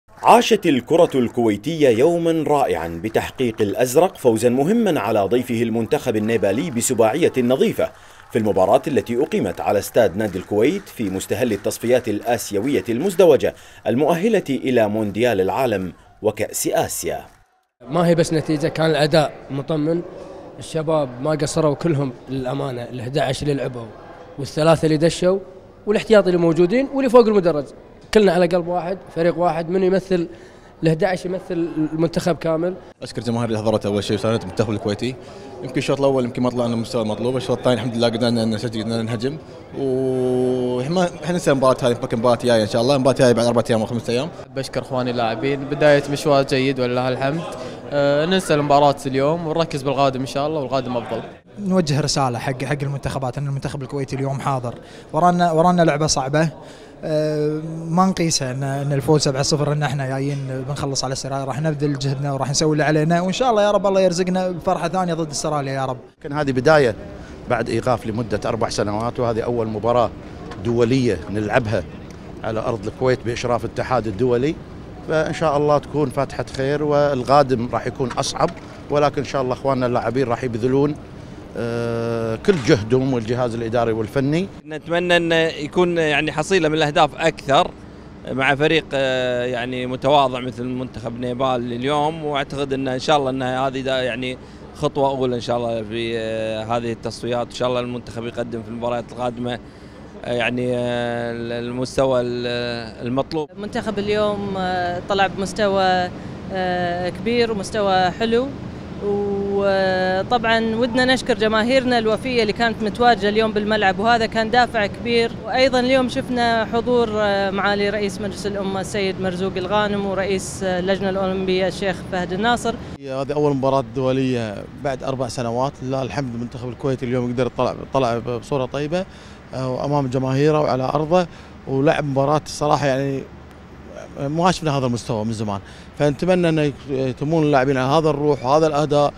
«الأزرق» يمطر شباك نيبال بسباعية نظيفة
هاني سيف الدين - القبس الإلكتروني

عاشت الكرة الكويتية أمسية رائعة بتحقيق «الأزرق» فوزاً كبيراً على ضيفه المنتخب النيبالي بسباعية نظيفة، في المباراة التي أقيمت على إستاد نادي الكويت، في مستهل مشواره بالتصفيات الآسيوية المزدوجة، المؤهلة إلى مونديال قطر 2022، وكأس آسيا.